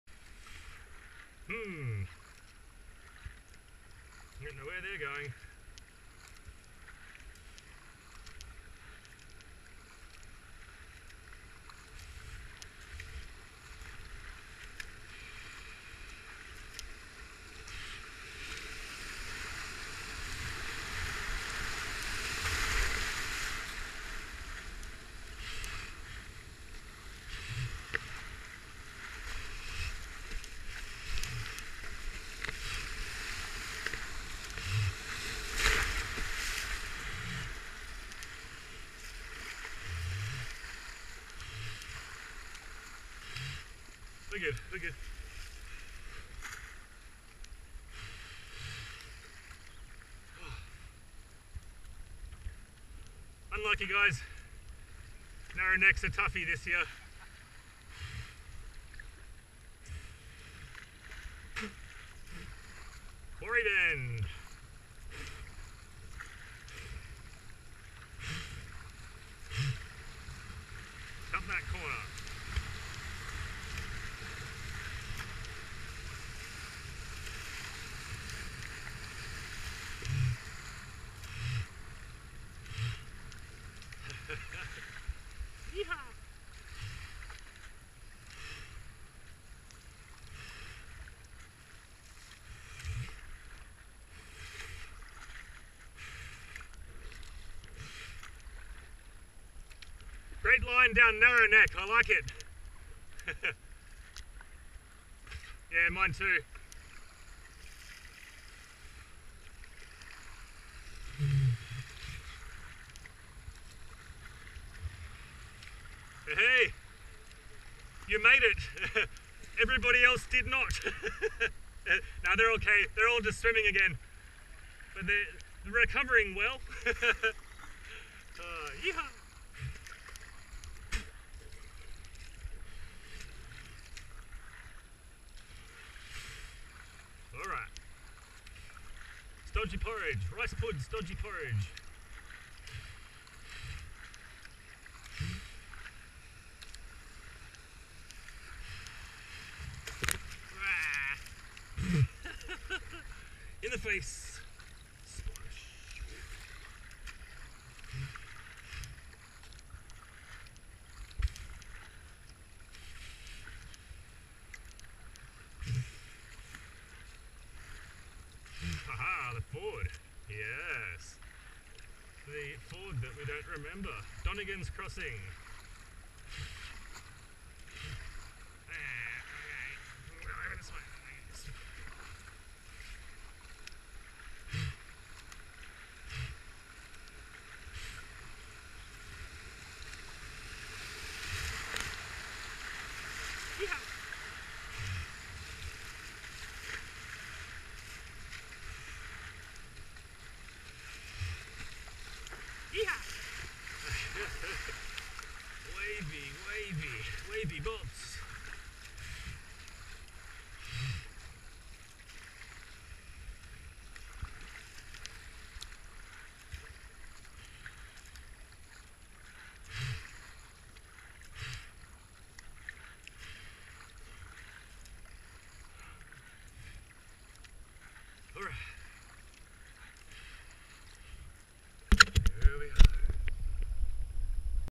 Avon Descent 2024 - 14 Day2 Narrow Neck to Quarry Bend
More waves, yum!

0:15 - Narrow Neck
0:30 - hard second part of Narrow Neck featuring camera splashing wave
1:08 - Quarry Bend
1:40 - my Dad and brother in the blue double kayak

Water level at Walyunga was 1.2m in the early morning and about 1.1m when we went thru